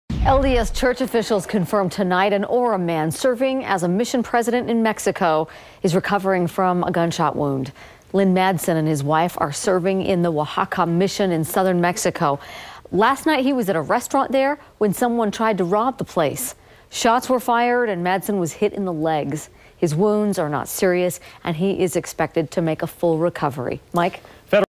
LDS mission president from Orem shot during robbery in Mexico
Lynn Madsen, a former Orem resident and Utah business owner serving as president of the LDS Church's Mexico Oaxaca Mission, was shot in the legs on Monday during an assault at a Subway restaurant in Mexico.

news,traffic,weather,sports,classifieds,cars,jobs,homes,television,radio,salt lake,utah,local